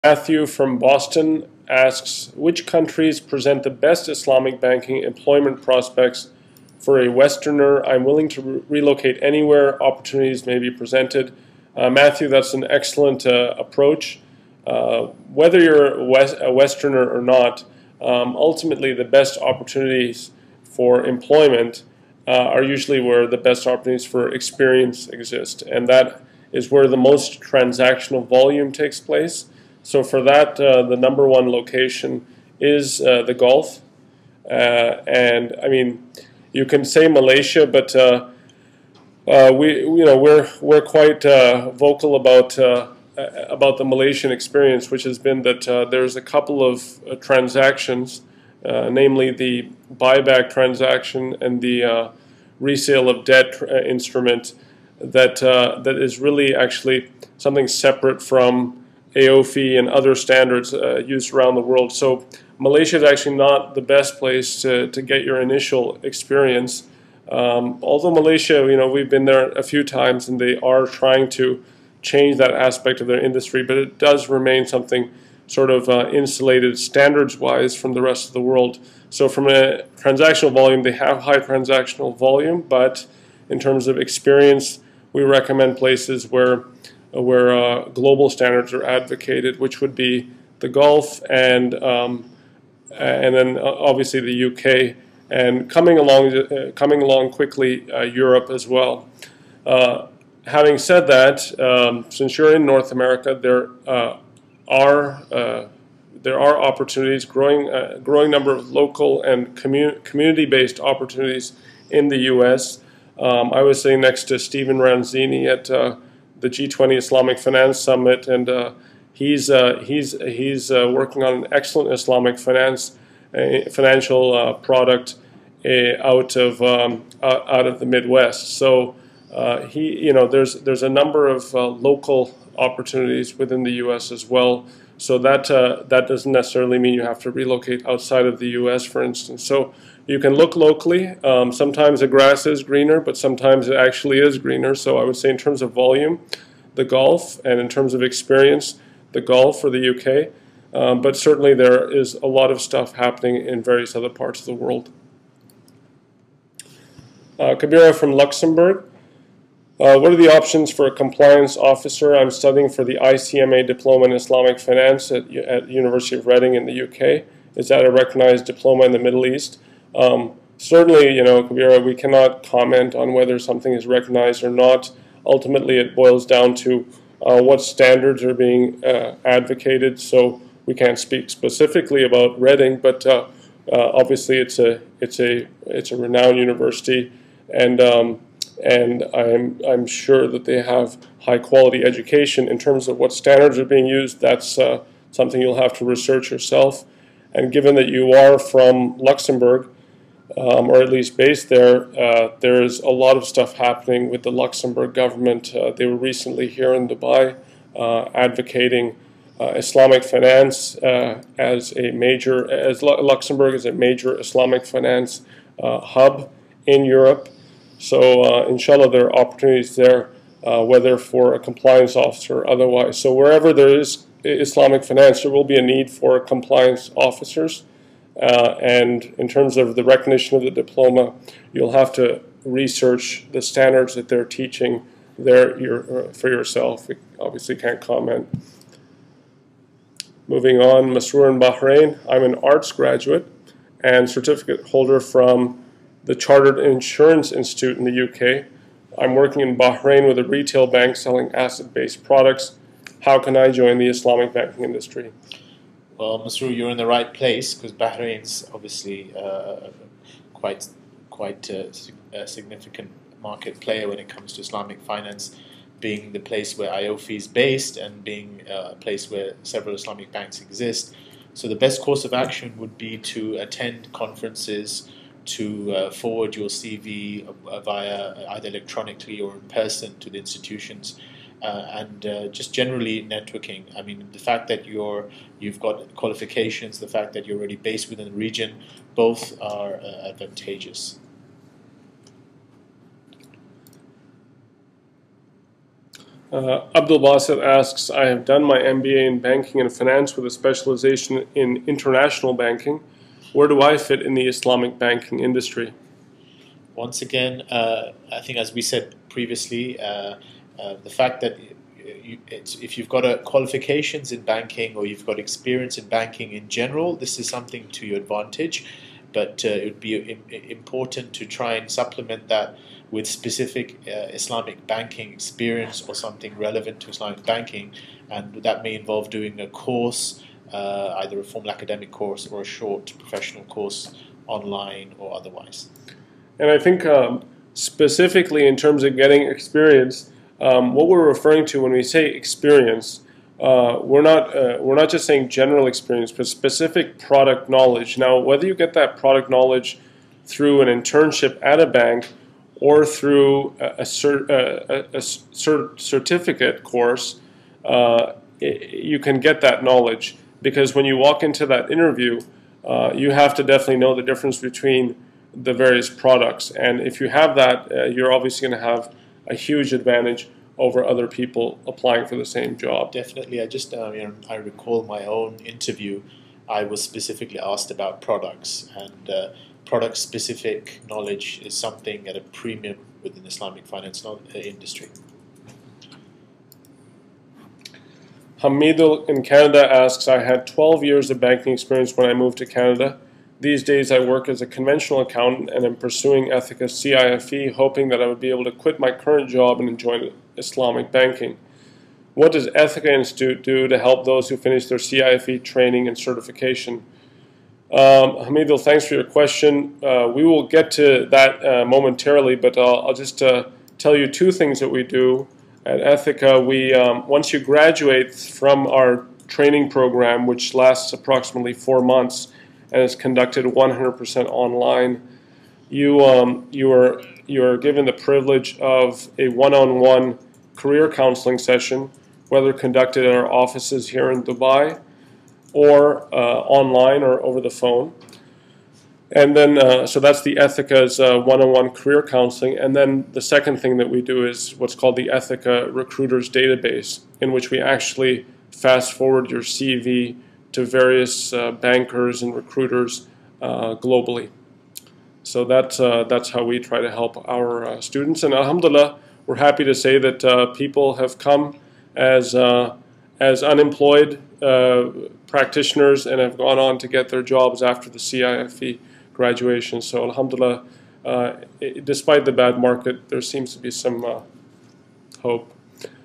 Career Options In Islamic Banking And Finance - Part 5
Attendee (Matthew): Which countries present the best Islamic banking employment prospects for a westerner? I'm willing to relocate anywhere opportunities may be presented.

Atif: Matthew that's an excellent approach whether you're a westerner or not, ultimately the best opportunities for employment are usually where the best opportunities for experience exist and that's where the most transactional volume takes place so for that the number one location is the Gulf. I mean you can say Malaysia but we are quite vocal about the Malaysian experience which has been that there are a couple of transactions namely the buy-back transaction and the resale of debt instrument that is really actually something separate from AAOIFI and other standards used around the world. So Malaysia is actually not the best place to get your initial experience. Although we've been there a few times, they are trying to change that aspect of their industry but it does remain something insulated standards-wise from the rest of the world. They have high transactional volume but in terms of experience we recommend places where global standards are advocated which would be the Gulf and obviously the UK, and coming along quickly, Europe as well. Having said that, since you're in North America there are growing number of local and community based opportunities in the US, I was sitting next to Stephen Ranzini at the G20 Islamic finance summit and he is working on an excellent Islamic financial product out of the mid-west so there's a number of local opportunities within the US as well. So that doesn't necessarily mean you have to relocate outside of the US for instance. So you can look locally, sometimes the grass is greener but sometimes it actually is greener so I would say in terms of volume, the Gulf, in terms of experience the Gulf or the UK but certainly there is a lot of stuff happening in various other parts of the world.

Attendee (Kabira): What are the options for a compliance officer, I'm studying for the ICMA diploma in Islamic finance in the University of Reading in the UK is that a recognized diploma in the Middle East?

Atif: Certainly you know Kabira we cannot comment on whether something is recognized or not. Ultimately it all boils down to what standards are being advocated so we can't speak specifically about Reading but obviously it's a renowned university and I'm sure that they have high quality education. In terms of what standards are being used, that's something you'll have to research yourself and given that you are from Luxembourg or at least based there, there is a lot of stuff happening with the Luxembourg government . They were recently here in Dubai advocating Luxembourg as a major Islamic finance hub in Europe, so there are opportunities there whether for a compliance officer or otherwise. So wherever there is Islamic finance there will be a need for compliance officers, in terms of the recognition of the diploma you'll have to research the standards that they're teaching there for yourselves. Obviously we can't comment.

Yusuf: Definitely, I recall my own interview, I was specifically asked about products and product specific knowledge is something at a premium within the Islamic finance industry.

Attendee (Hamidul): I had 12 years of banking experience when I moved to Canada, these days I work as a conventional accountant and I'm pursuing Ethica's CIFE™, hoping that I'll be able to quit my current job and join Islamic banking. What does Ethica™ Institute do to help those who finish the CIFE™ training and certification?

Atif: Hamidul thanks for your question. We will get to that momentarily but I'll just tell you two things that we do. At Ethica™ once you graduate from our training program which lasts approximately 4 months, and is conducted 100% online, you are given the privilege of a 1-on-1 career counseling session whether conducted at our offices here in Dubai or online or over the phone. So that's the Ethica's™ 1-on-1 Career Counseling. And then the second thing that we do is what we call Ethica's™ Recruiters Database in which we actually fast forward your CV to various bankers and recruiters globally. So that's how we try to help our students and we're happy to say that people have come as employed practitioners and have gone on to get their jobs after the CIFE™ graduation. So despite the bad market there seems to be some hope.